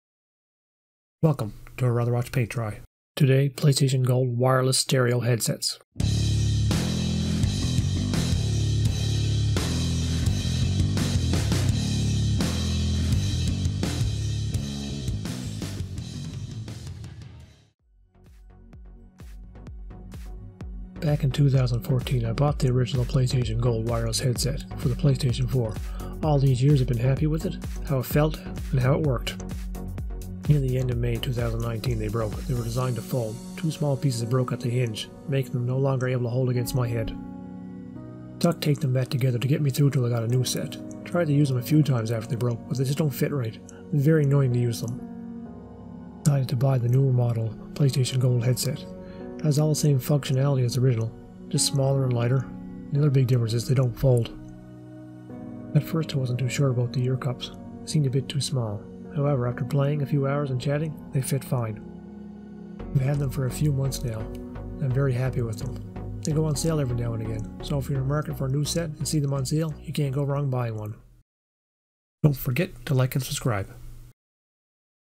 PlayStation Gold Wireless Stereo Headsets Quick Review
Have two sets of PlayStation Gold Wireless Stereo Headsets.  The original one and the redesigned model.  Like the original a lot until the hinge broke one side.  That is when I picked up the newer model.  At first didn’t like them as much.  However after a new months. Realized they are much better.